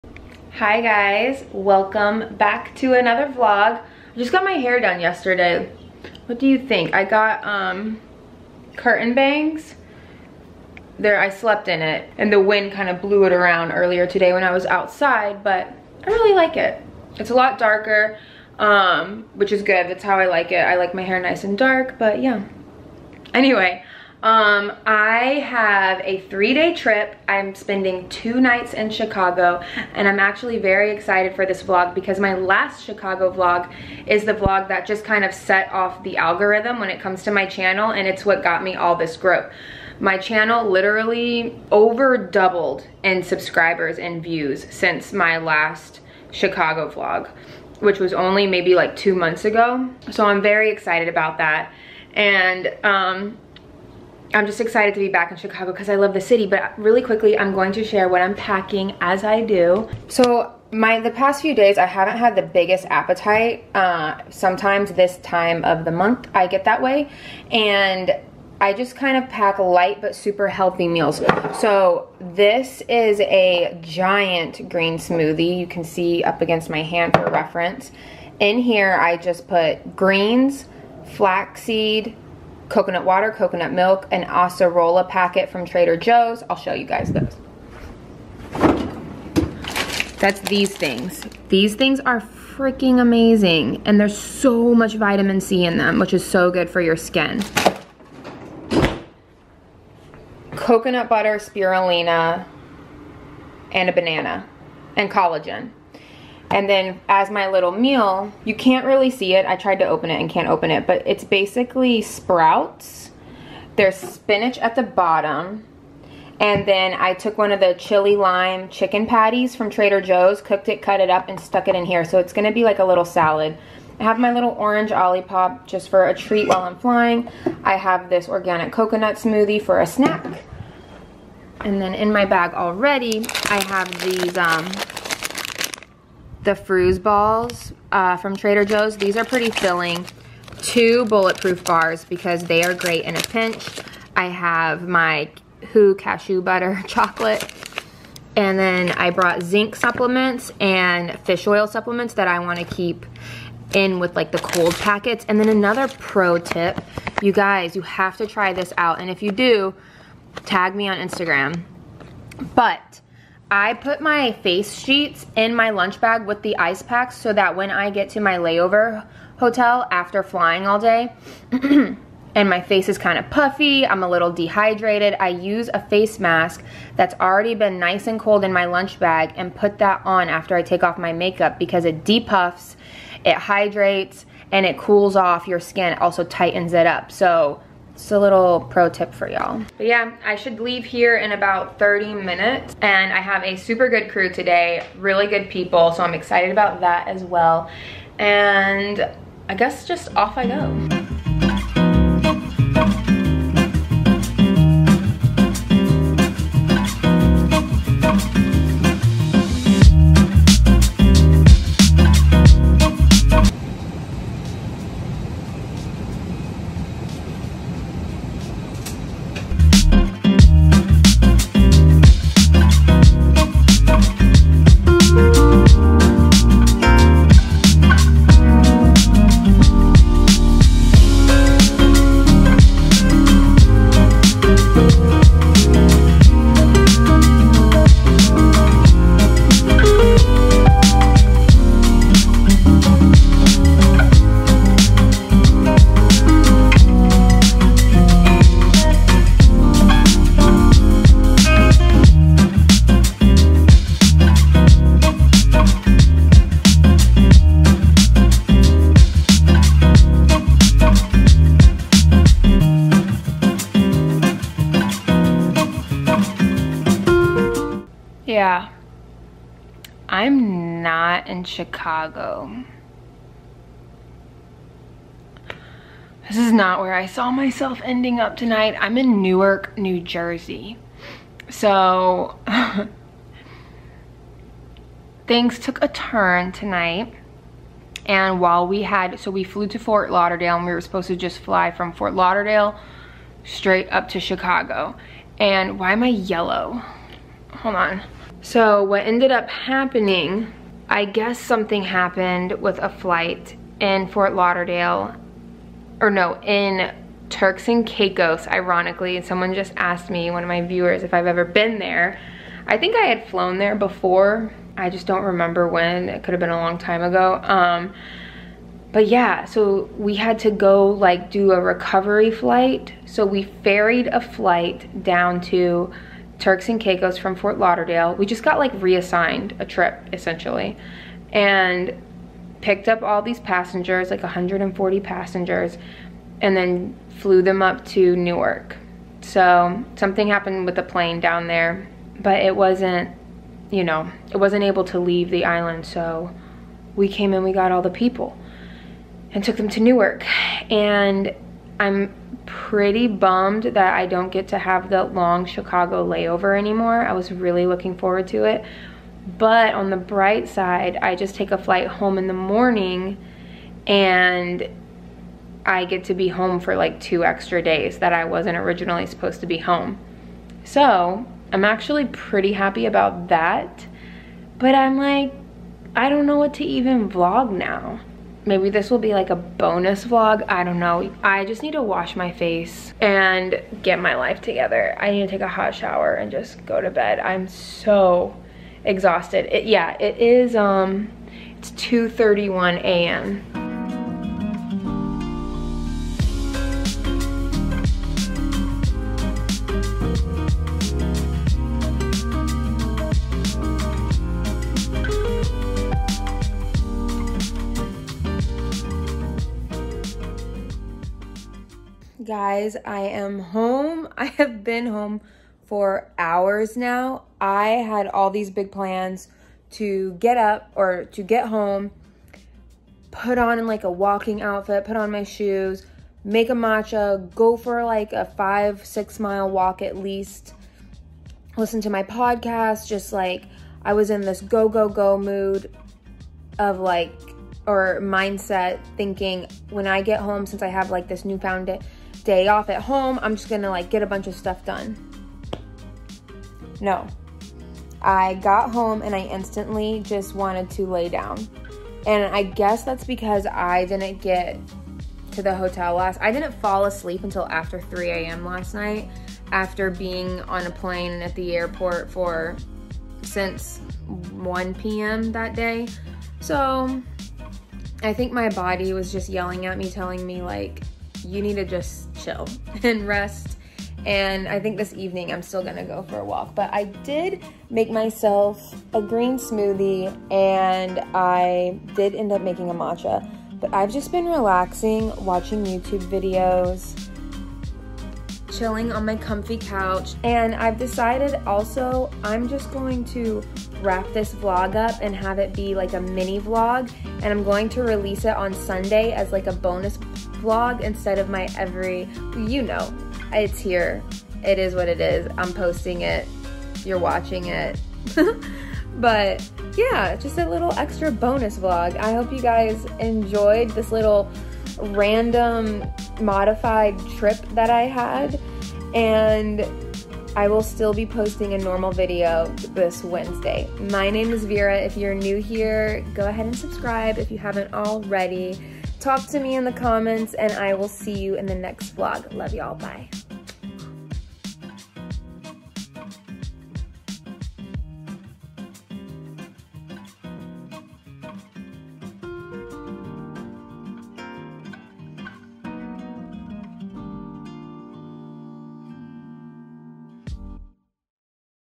Flight Attendant Vlog 🥴
Welllll that didn't go as planned.  BUT that's life in the travel industry. Being a flight attendant definitely has its days and unpredictability sometimes. It keeps the job interesting I'll give it that. But I am grateful to have a little more time at home to relax and get ready for my next trip!